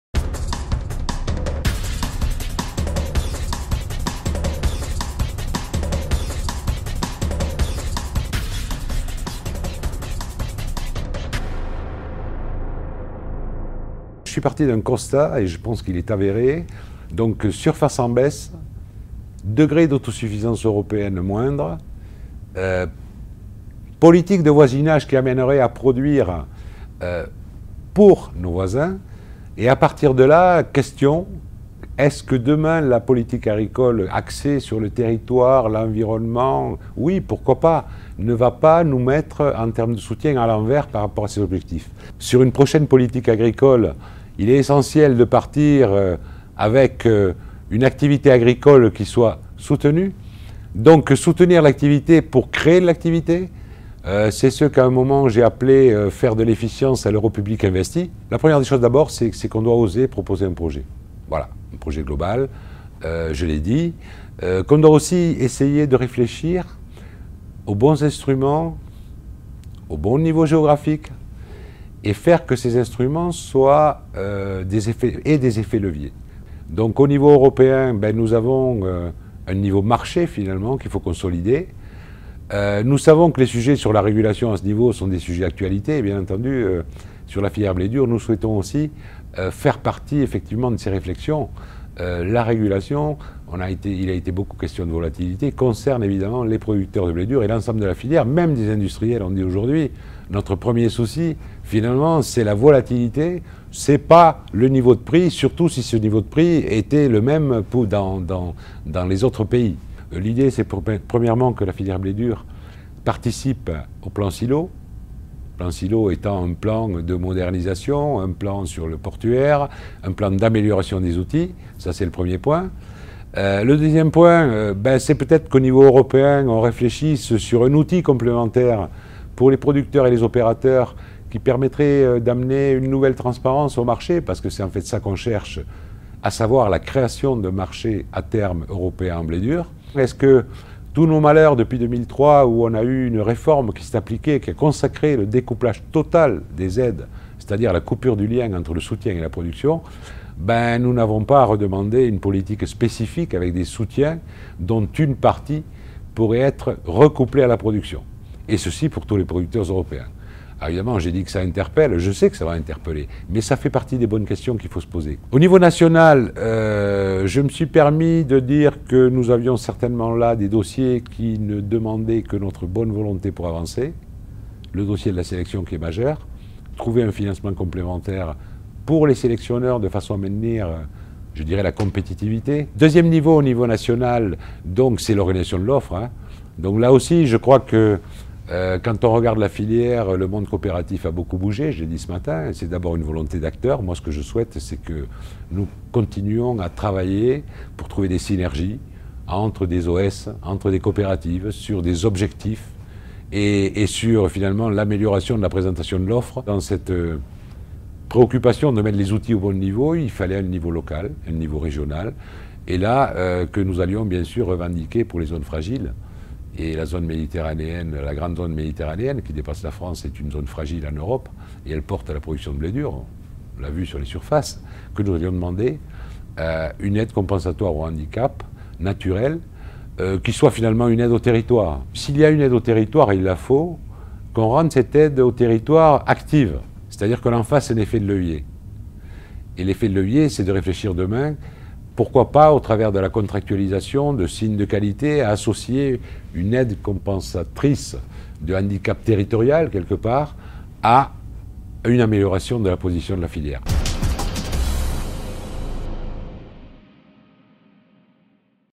PAC 2013 : les propositions de la filière blé dur - ARVALIS-infos.fr
L'année 2013 sera marquée par la nouvelle Politique Agricole Commune. Dans cette perspective, Jean-François Gleizes, présent à la 13e Journée nationale filière blé dur organisée par ARVALIS - Institut du végétal, a proposé différents axes de développement pour la filière, afin de garantir la compétitivité de ce secteur malgré la conjoncture actuelle : volatilité des prix, pressions réglementaires...

Retrouvez toute l'info technique des grandes cultures et des fourrages en vous abonnant à notre lettre ARVALIS-infos.fr